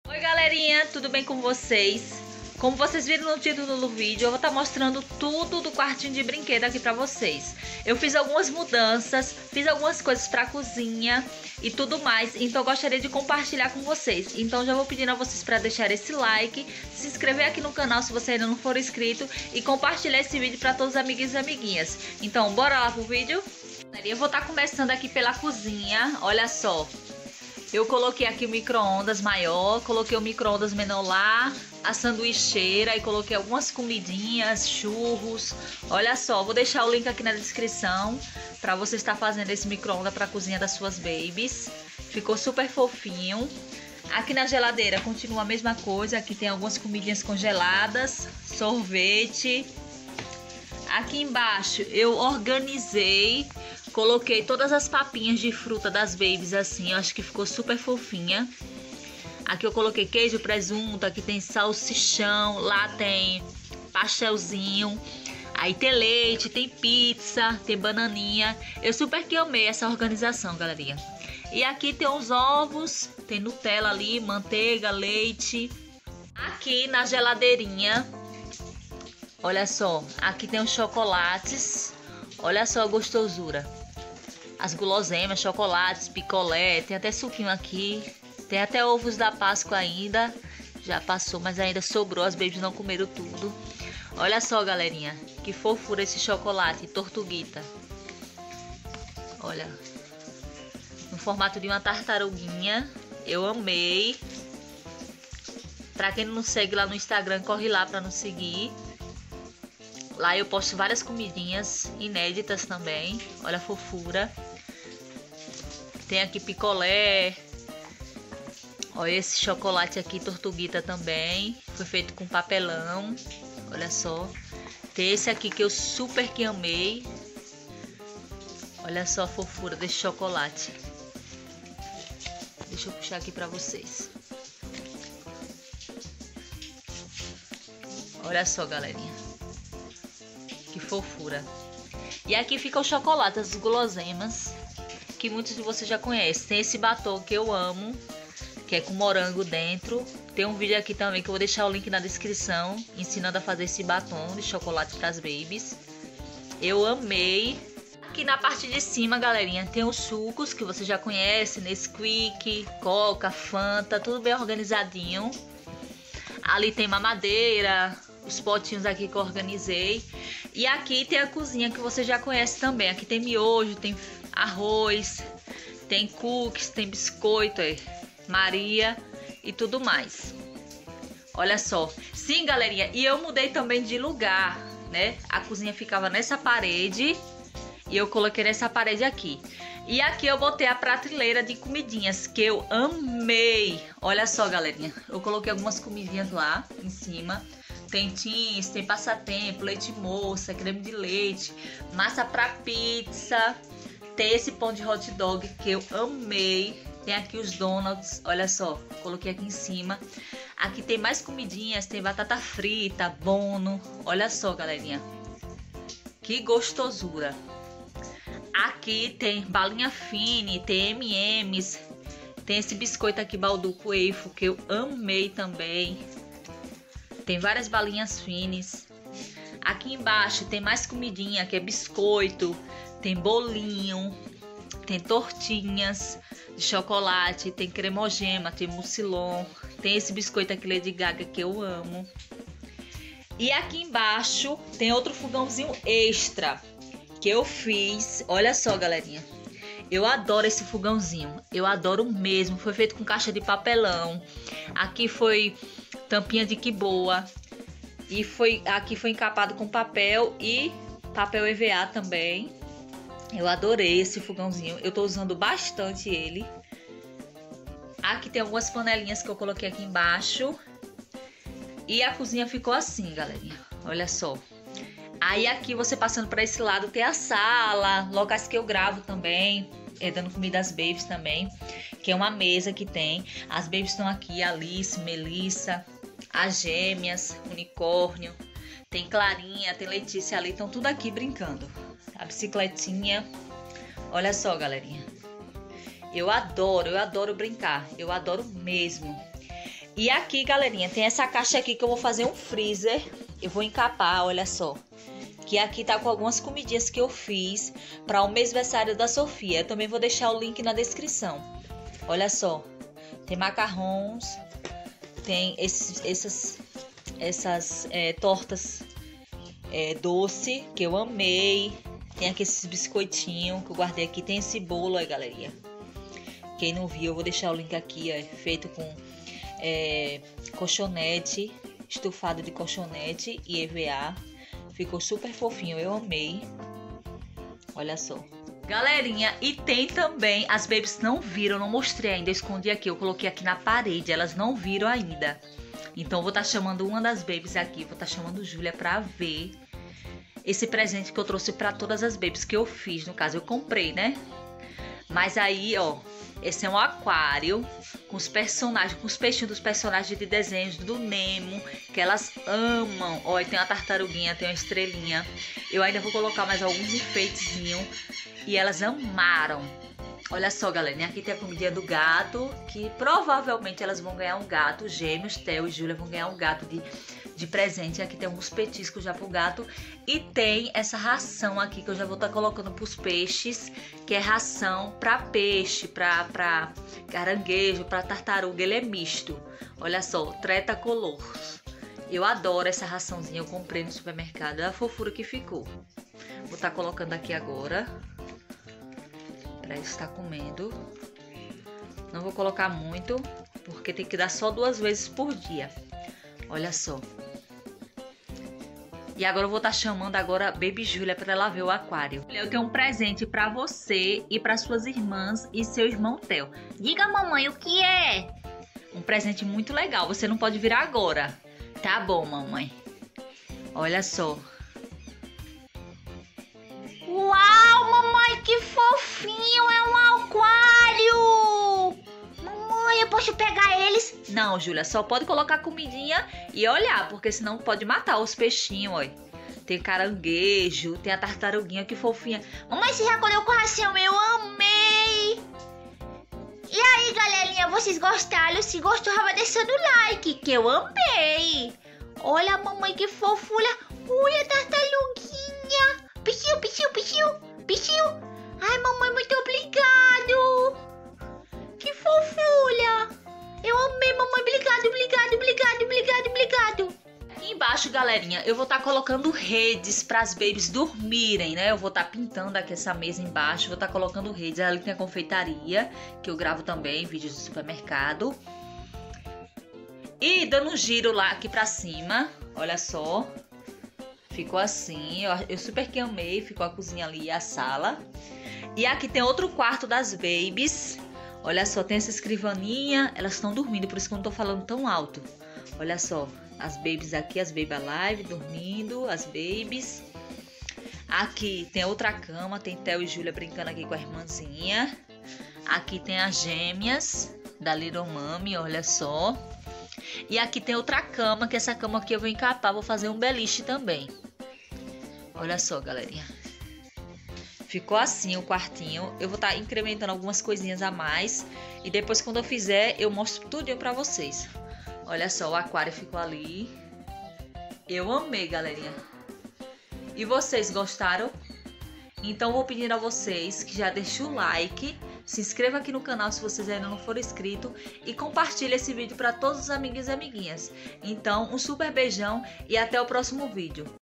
Mostrando tudo do meu quartinho de brinquedos. Atualizado.
Olá galerinha tudo bem com vocês? no vídeo de hoje vocês vão ver tudo do quartinho de brinquedos atualizado eu fiz algumas mudanças fiz algumas coisinhas para cozinha das babies então fica até o final do vídeo para não perder nadinha do que eu vou mostrar um super beijão❤️